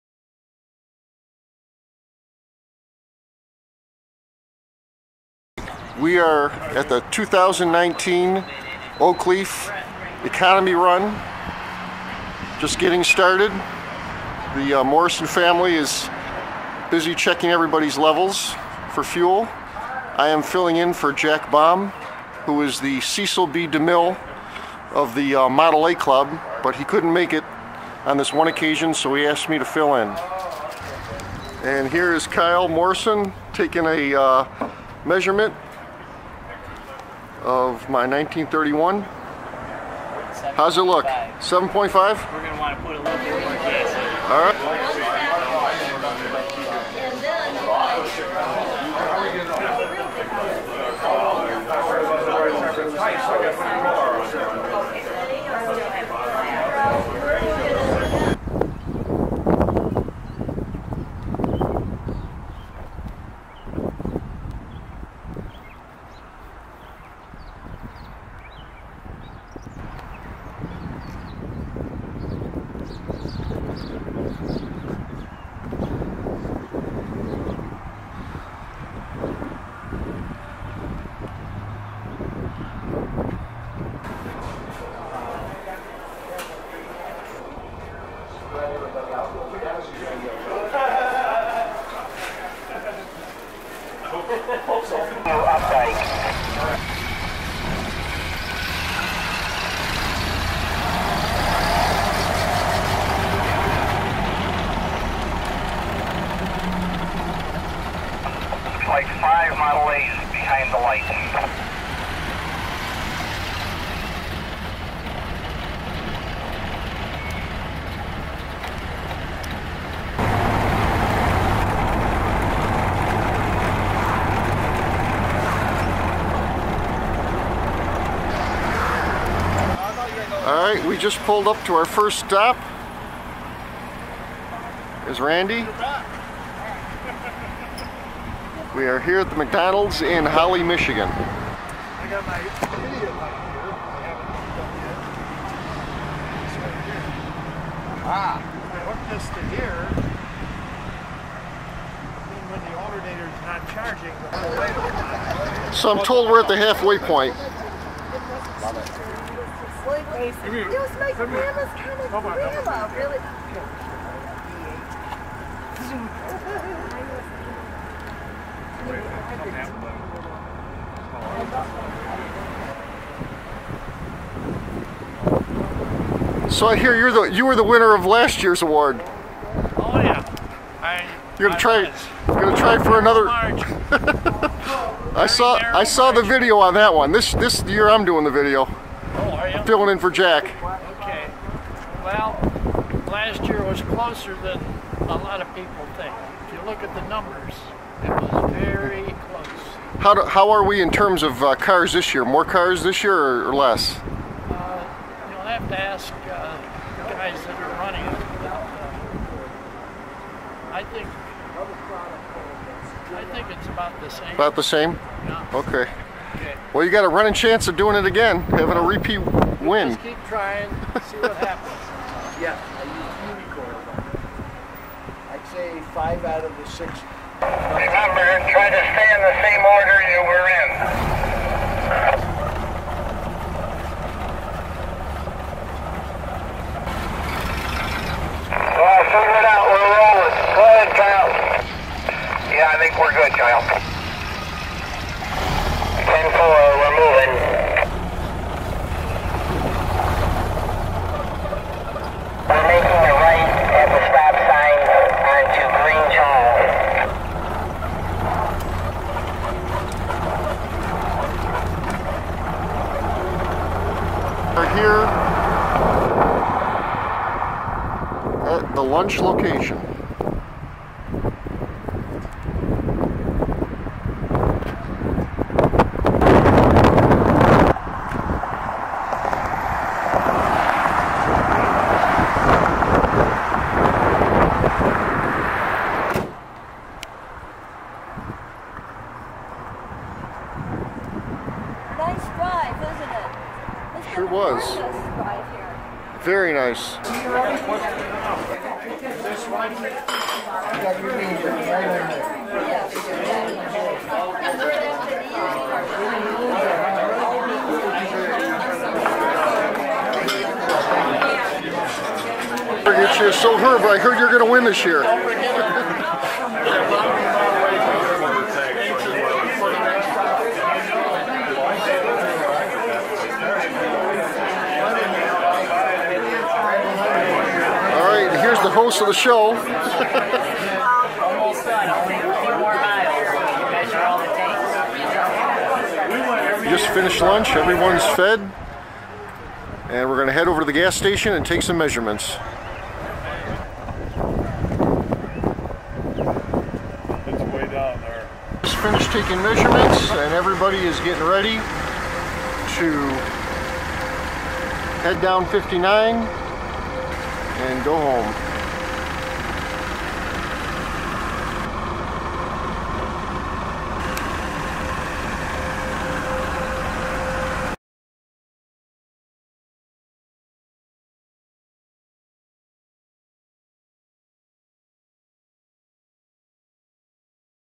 Economy Run 2019
Model A fuel economy run 2019  Winner had 22 MPG and Gas Guzzler got 12 MPG